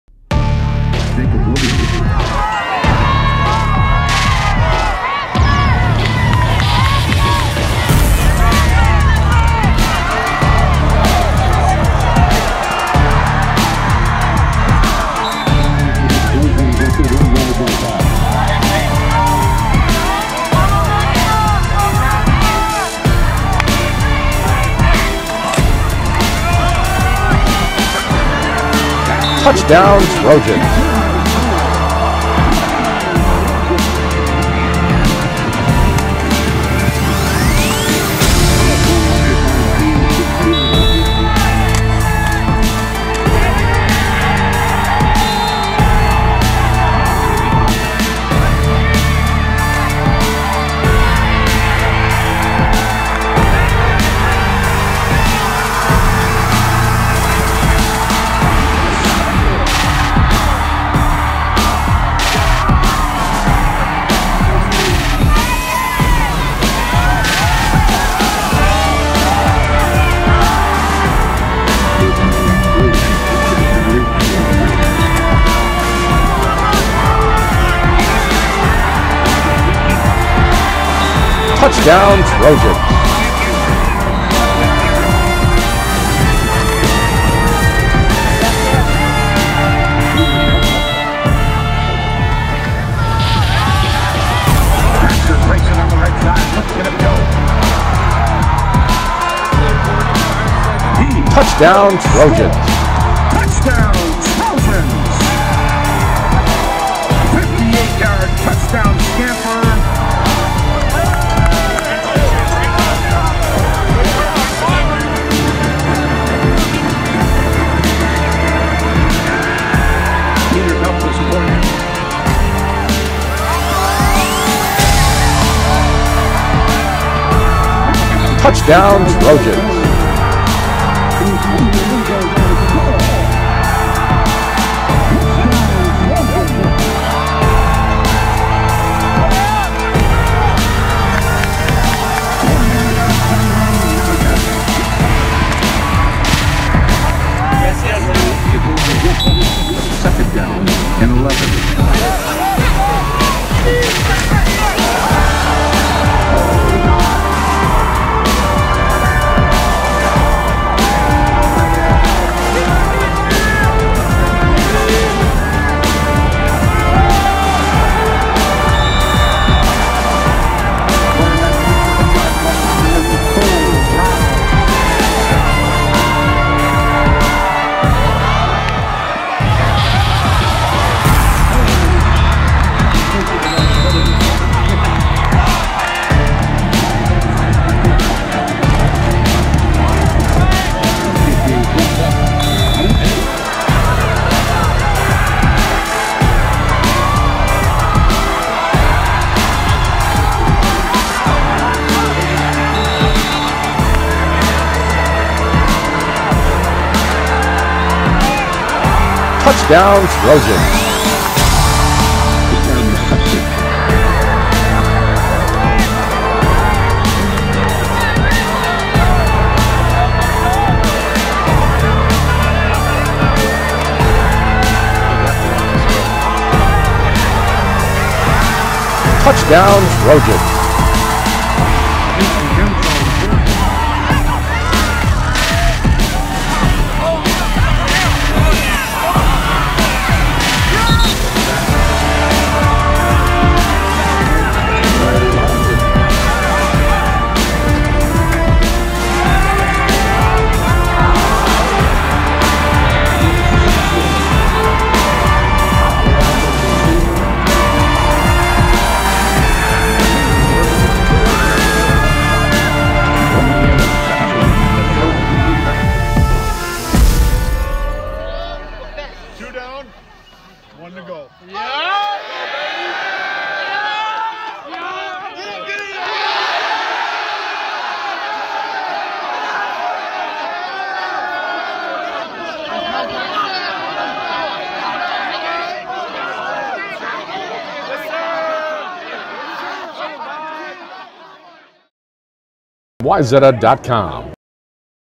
Wayzata 56, East Ridge 20
Wayzata Football highlights as the Trojans roll the East Ridge Raptors. Sponsor